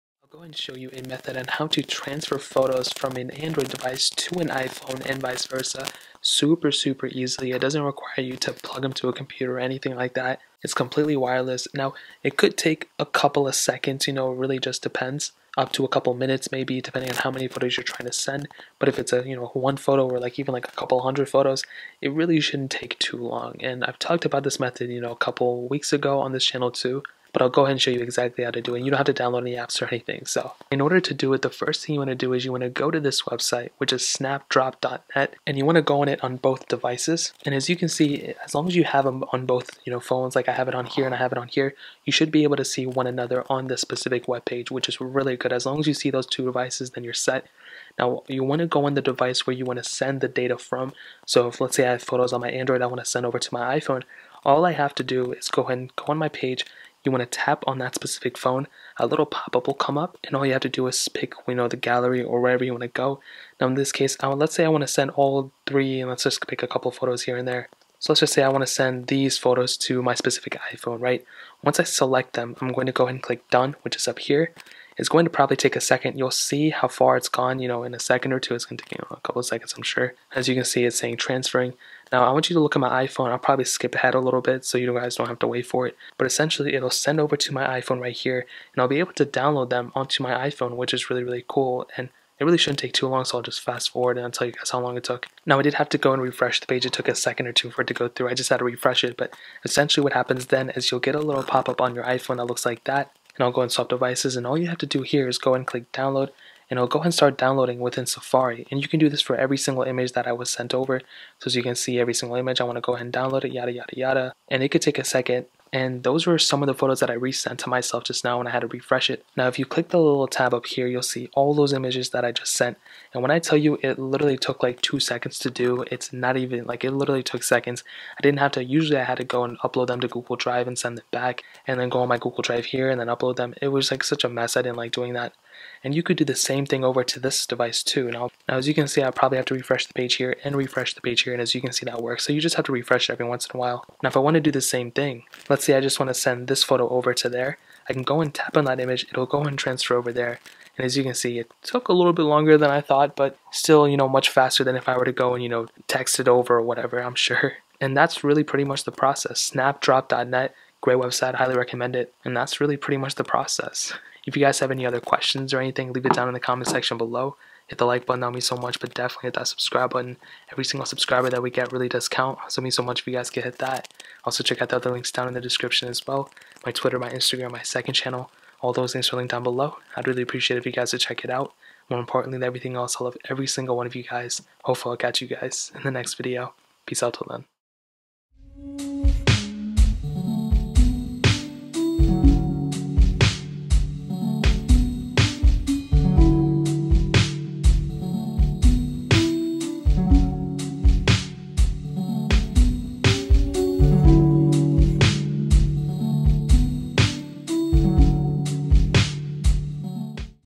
How To Transfer Photos From Android To iPhone EASILY! (2020)
Here is exactly how to transfer photos from and android to and iphone (and vice versa) super easily without downloading any apps!